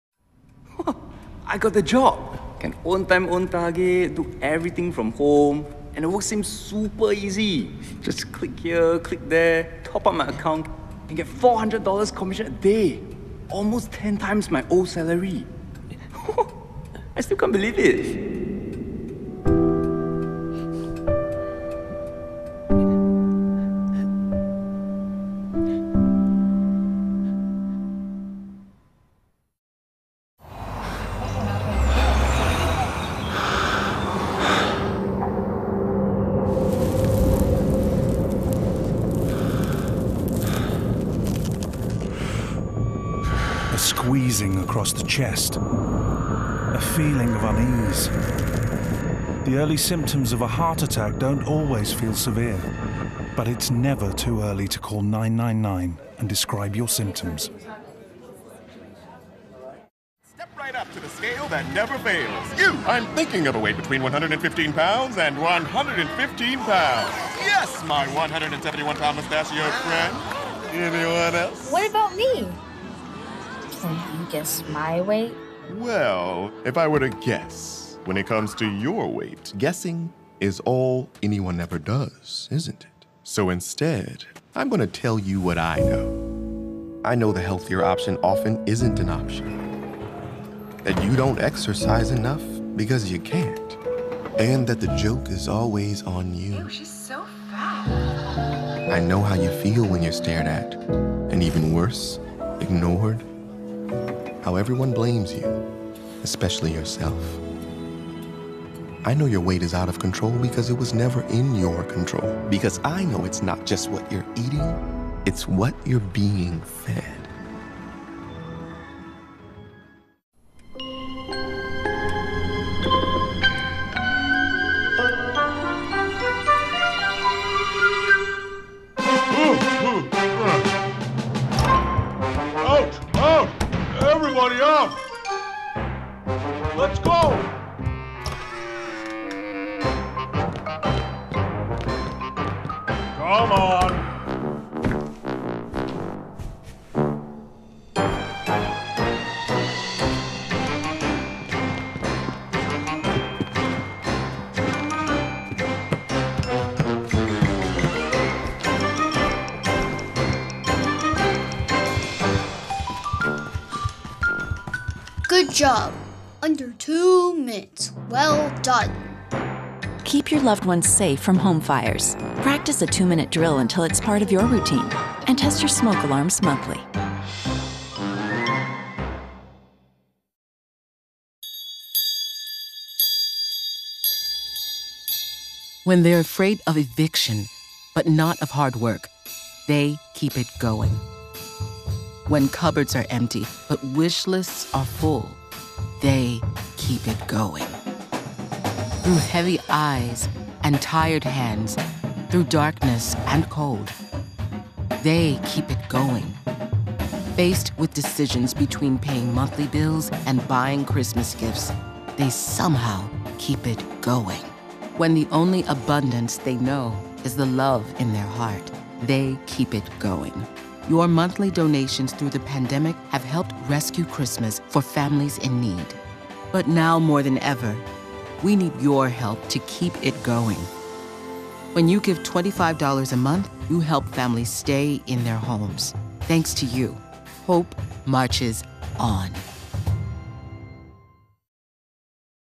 Public Safety Adverts #2 | NHS, American Red Cross, The Salvation Army
Public Safety Adverts #2

[00:00] NCPC - The New Job (Singapore) (2022)
[00:30] NHS - Don't dismiss the early signs of a heart attack (UK) (2022)
[01:00] Pennington Biomedical - Weight Guesser (USA) (2021)
[02:00] American Red Cross - The Great Escape (USA) (2021)
[03:00] The Salvation Army - Keep It Going (USA) (2021)

Spotvee doesn't own any rights to these commercials.